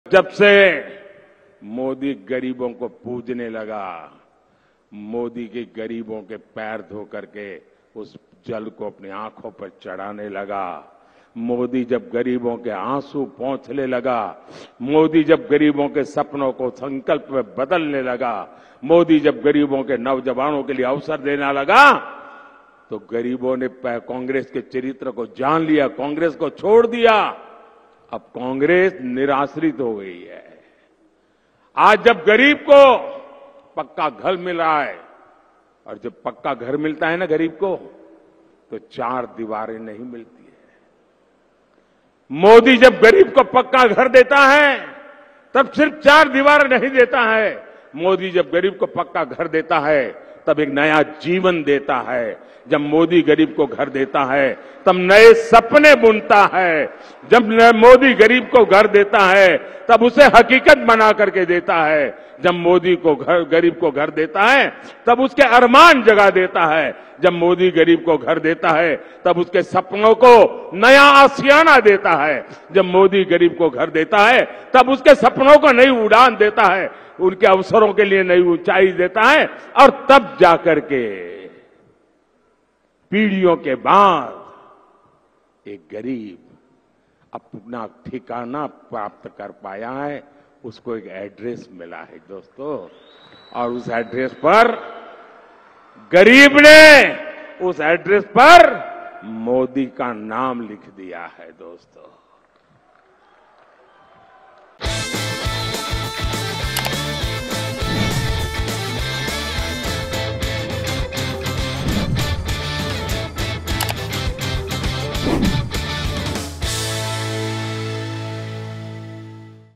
"Now That Poor Has An Address", PM Modi As He Elaborates How Poor Got Benefited Under His Leadership
Prime Minister Narendra Modi, in a recent rally, emphasized the transformative impact of his policies on the lives of the poor. With the evocative phrase, "Now That Poor Has An Address," PM Modi elucidated how his administration has prioritized the welfare of the underprivileged. He highlighted various initiatives and schemes aimed at empowering the economically marginalized sections of society, ranging from financial inclusion programs to housing and healthcare initiatives.